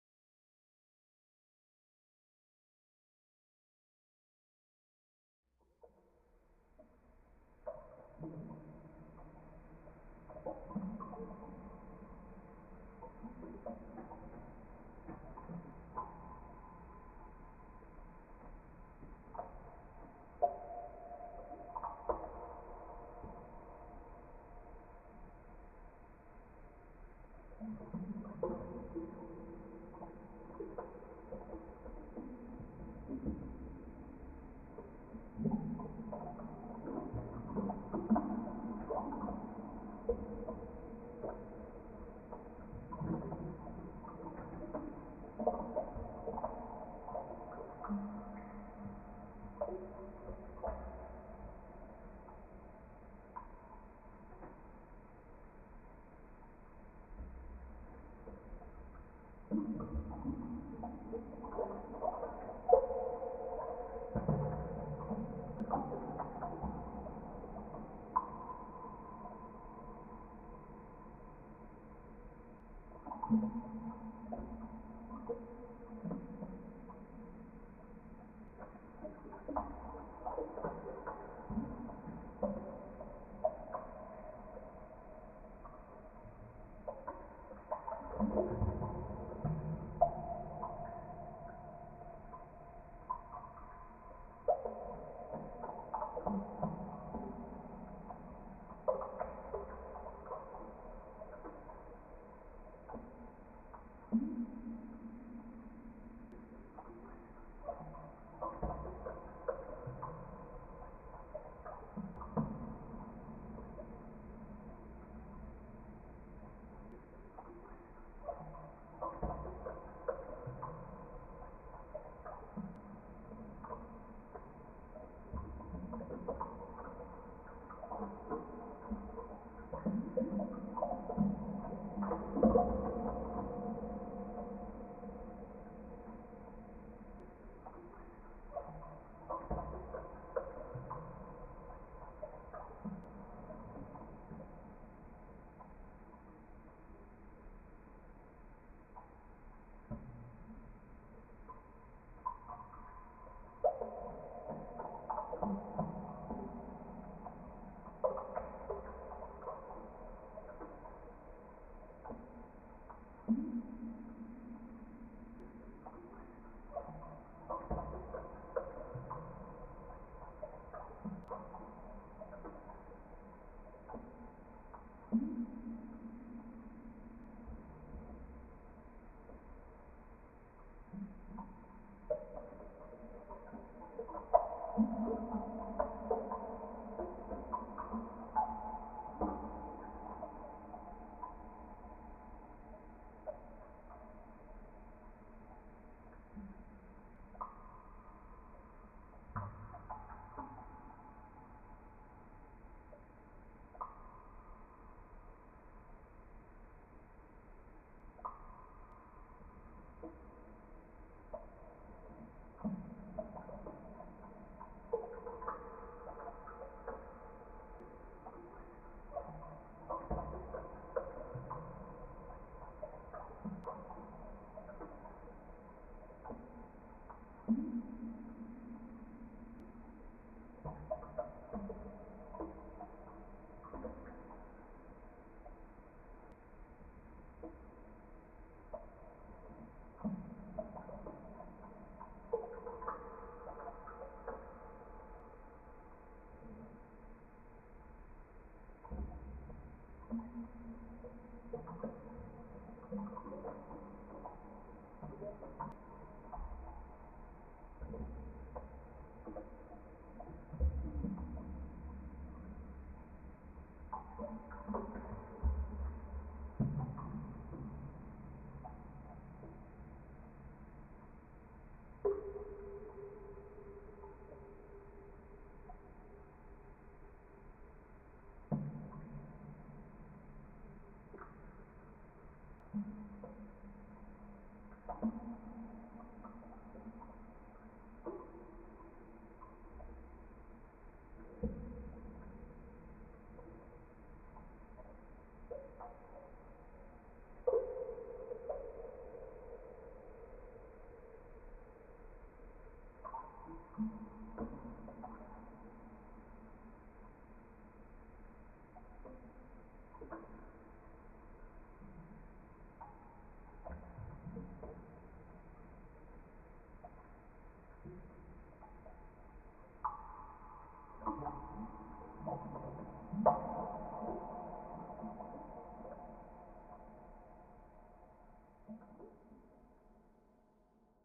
corpus
Corpus, di Sandro Pellarin e Silvia Lepore, fa parte di un'installazione che porta lo stesso titolo